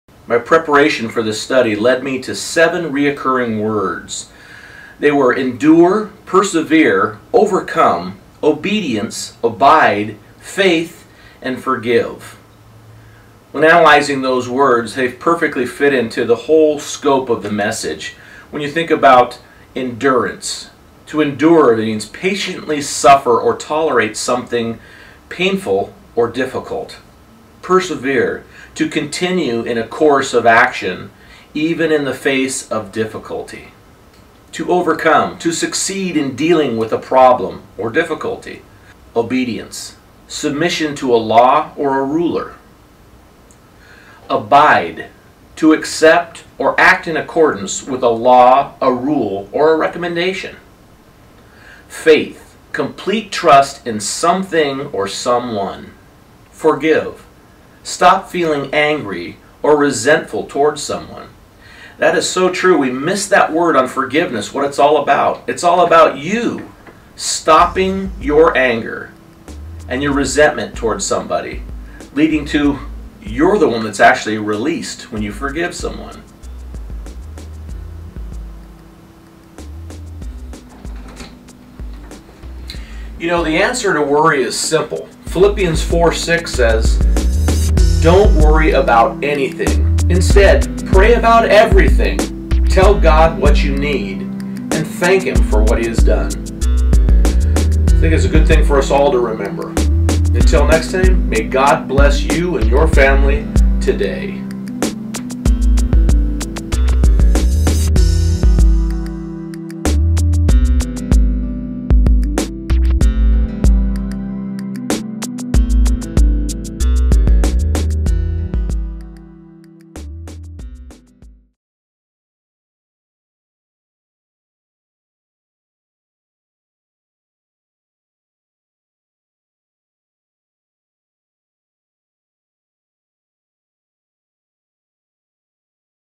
Stress and Joy Part 3 of 3
The conclusion in the discussion of stress and joy in the troubles times that we are in.